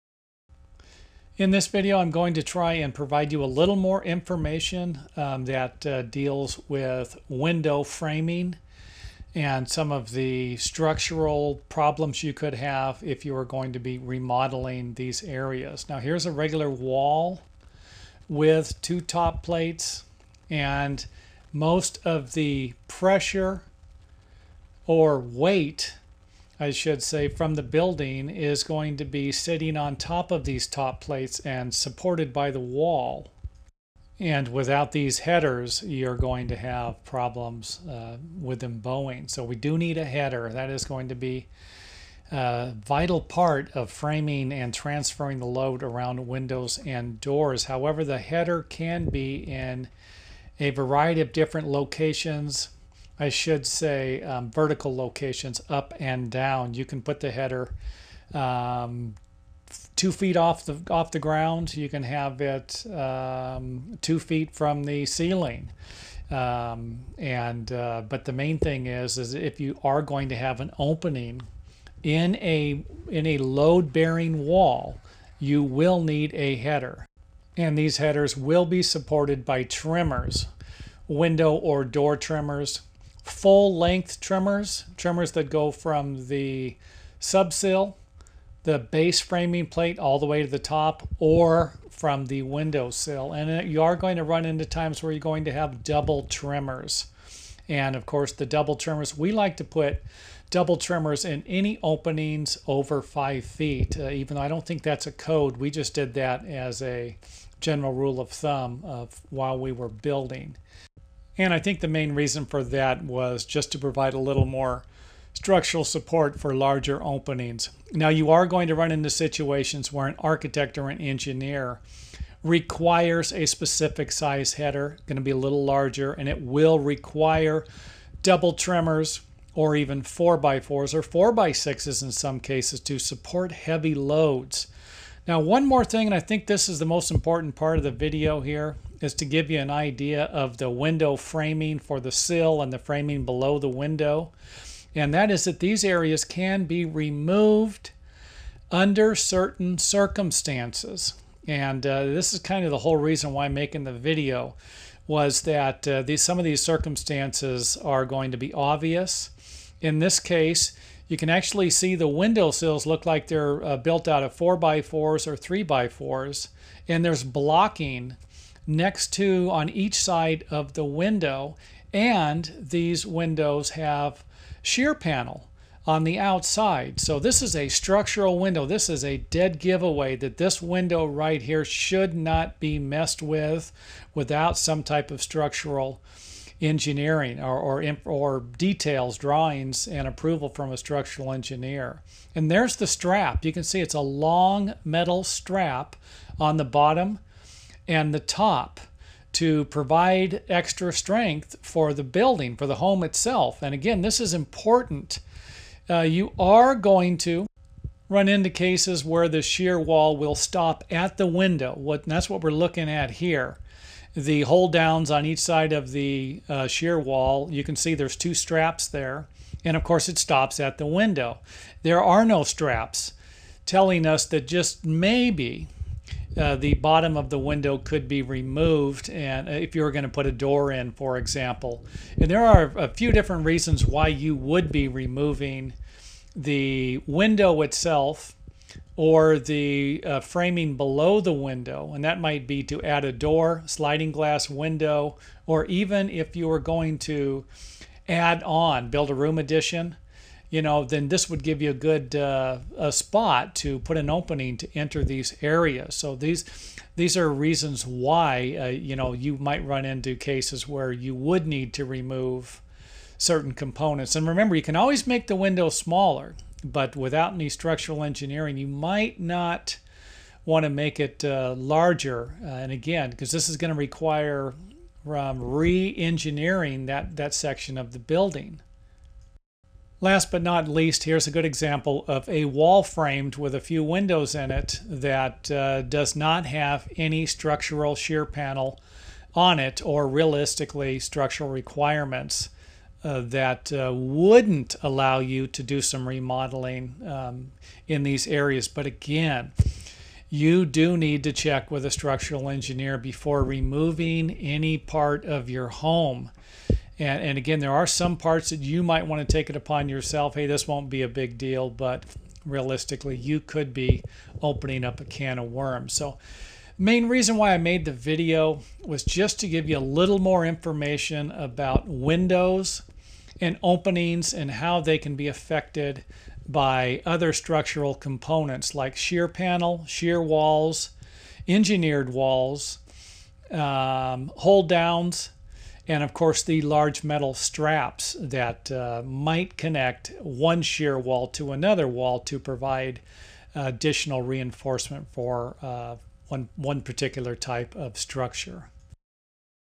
Window Framing - Structural Engineering and Home Building Part 5
Click on this link for more information about engineering, renovation and construction. This video will provide you with more information about load bearing walls, structural engineering and home building along the lines of window framing construction. If you're planning on remodeling any part of your home, make sure you watch as many of these helpful videos as you possibly can, before accidentally removing something you shouldn't have, like a load bearing wall.